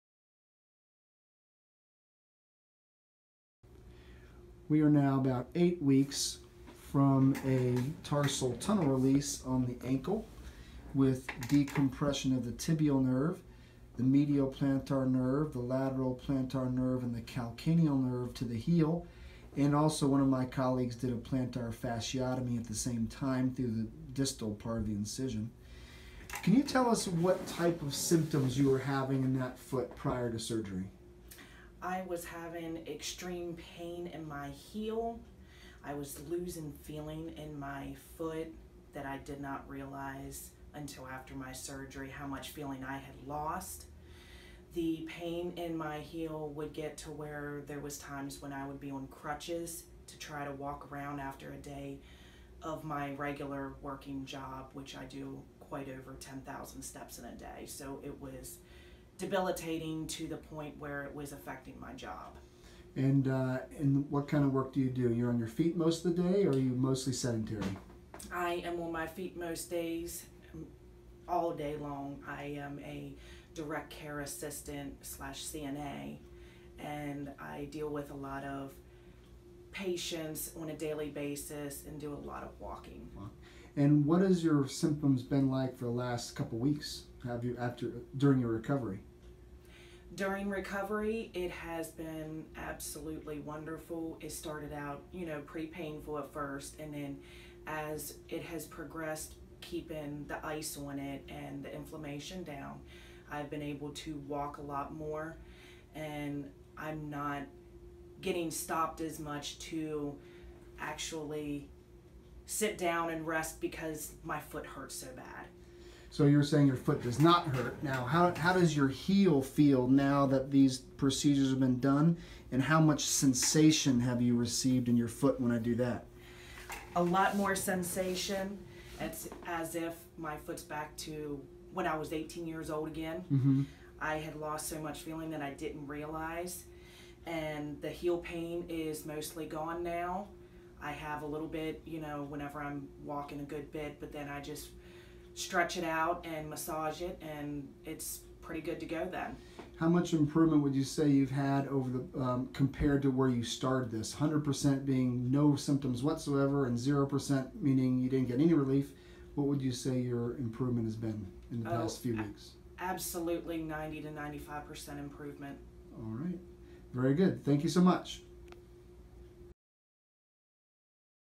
Eight Weeks Following Tarsal Tunnel Release | Baltimore Tarsal Tunnel Relief
We are now about 8 weeks from a Tarsal Tunnel release on the ankle with a deep impression of the Tibial Nerve, the Medial Plantar Nerve, the Lateral Plantar Nerve, and the Calcaneal Nerve. And also, one of my colleagues did a Plantar Fasciotomy at the same time to the distal part of the incision. Can you Tell us what kind of symptoms you were having in that foot prior to surgery? \n\n   \n\n “I was having extreme pain in my heel. I was losing feeling in my foot that I did not realize until after my surgery how much feeling I had lost. The pain in my heel would get to where there was times when I would be on crutches to try to walk around after a day of walking around at my regular day job which I do over 10,000 steps in a day.  It was debilitating to the point where it was affecting my job.” \n\n   \n\n "What kind of work do you do? Are you on your feet most of the day or are you sedentary? \n\n   \n\n “I am on my feet most days, all day long I am a Direct Care Assistant/CNA and I deal with a lot of patients on a daily basis and do a lot of walking.” \n\n   \n\n What have your symptoms been like for the last couple of weeks now during recovery? \n\n   \n\n “During recovery, it was been absolutely wonderful. It started out pretty painful at first and then as it has progressed, keeping the ice on it and keeping the inflammation down, I’ve been able to walk a lot more and I’m not getting stopped as much to actually sit down and rest because my foot does not hurt so bad.” \n\n   \n\n So you’re saying that your foot does not hurt? Now how does your heel feel now that these procedures have been done, and how much sensation do you feel in your foot when I do that? \n\n   \n\n “A lot more sensation as if my foots back to when I was 18 years old again.  I had lost so much feeling that  I didn’t realize and the heel pain is mostly gone now. I have a little bit when I’m walking but then I just stretch it out and massage it and its pretty good to go then.” \n\n   \n\n How much improvement would you say you’ve had compared to when you started this? 100% meaning no symptoms whatsoever, 0% meaning you didn’t get any relief . . . what would you say your improvement has been? \n\n   \n\n “Absolutely 90-95% improvement.” \n\n   \n\n Very good, thank you so much! \n